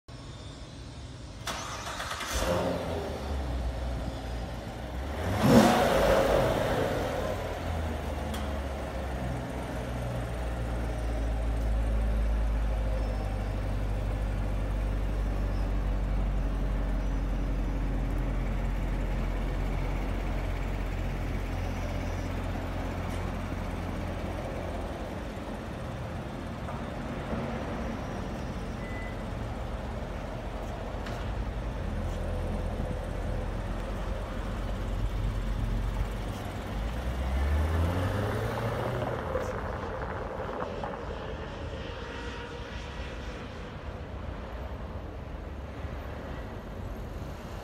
2016 Chevy Corvette Convertible - Lexus of Las Vegas
Drop the top and hit the strip in this gorgeous Corvette. Lexus of Las Vegas offers top of the line pre-owned vehicles in addition to our new Lexus models.